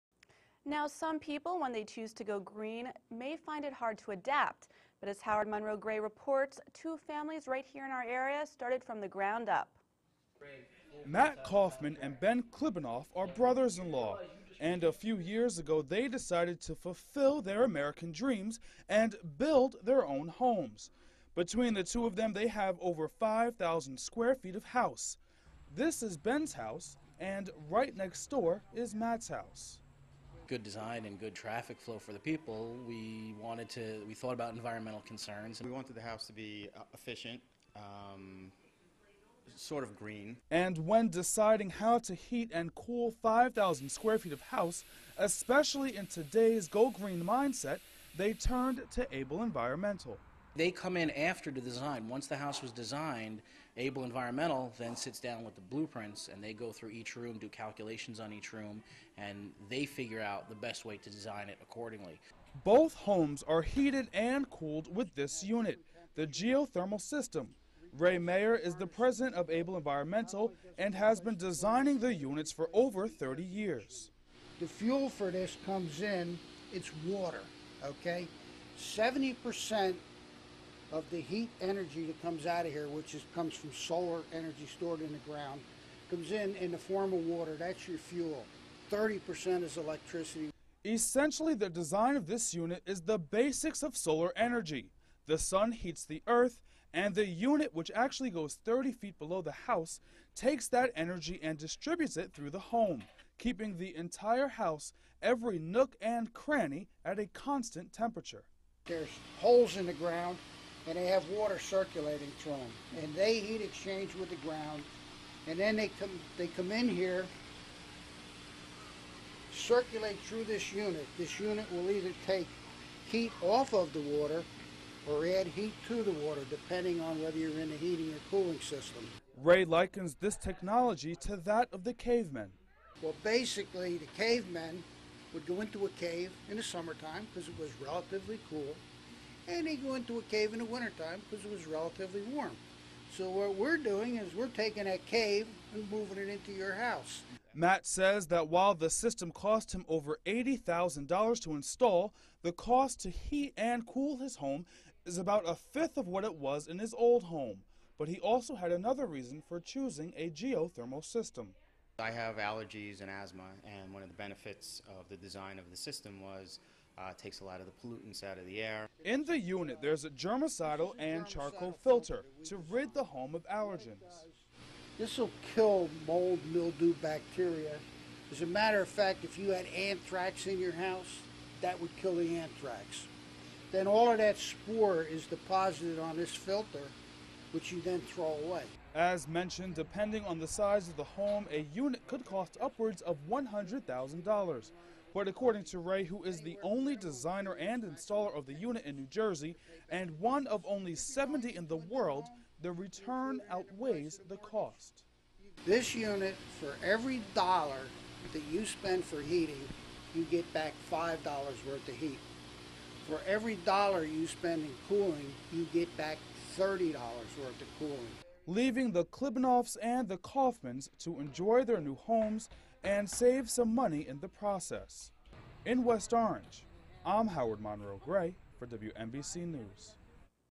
NJ Geothermal TV News Broadcast
The secret of solar powered alternative renewable energy and how NJ homeowners $ave thousand$ while keeping their homes comfortable, healthy and clean. Presented by WNBC TV and Able Environmental www.